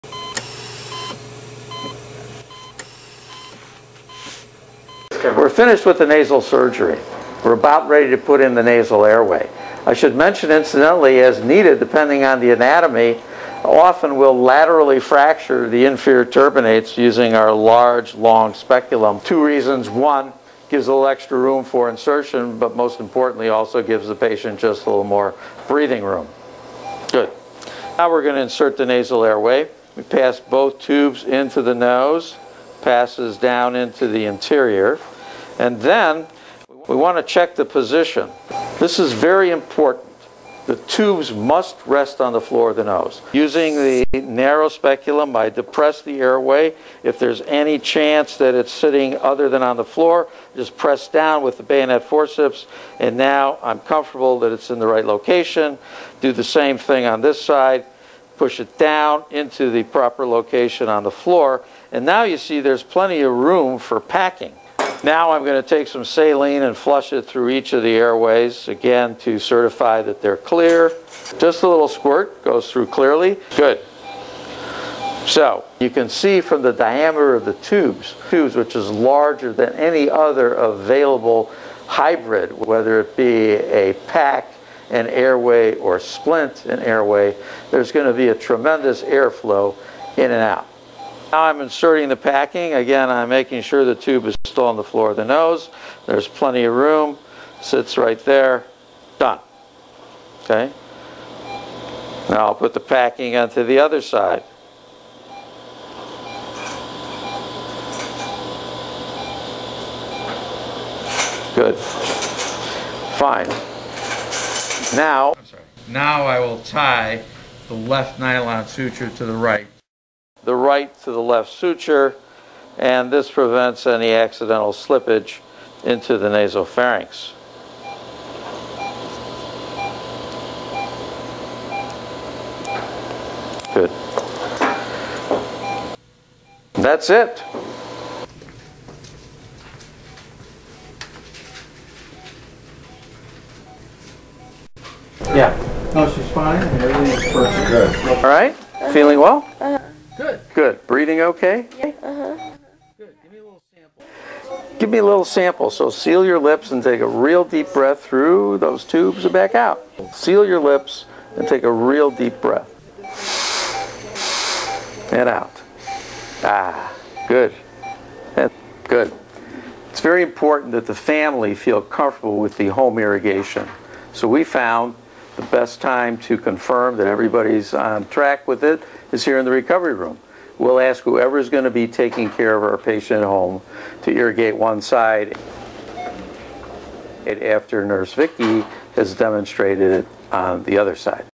Inserting the Nasal Airway System
Video featuring the insertion of the Reltok Clear-Flo Nasal Airway System